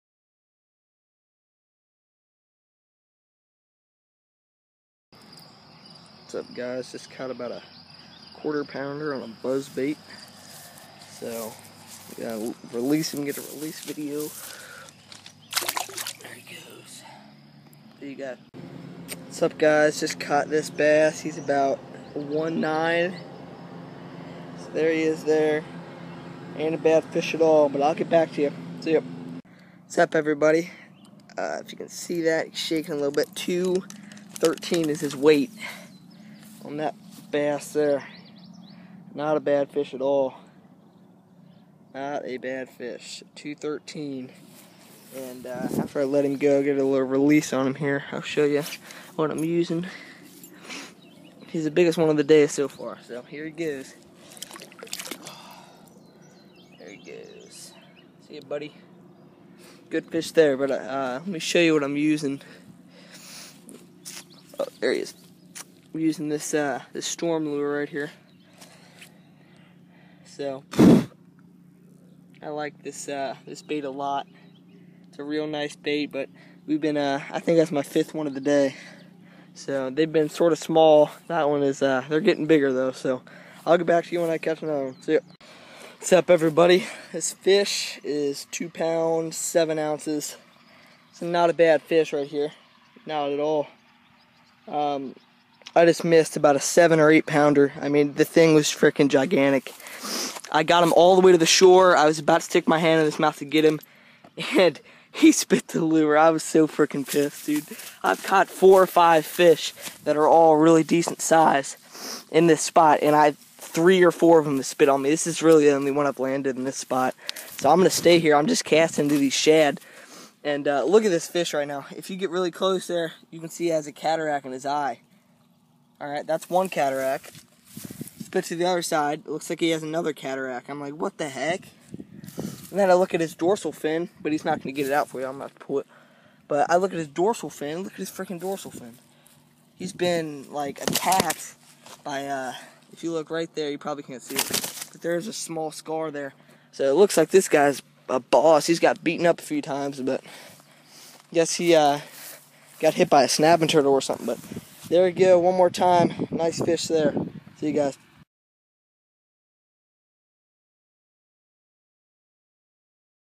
Summer Bass Fishing
i target post spawn bass and catch a few nice ones during the day. i didnt gget some of them on camera im sorry about that.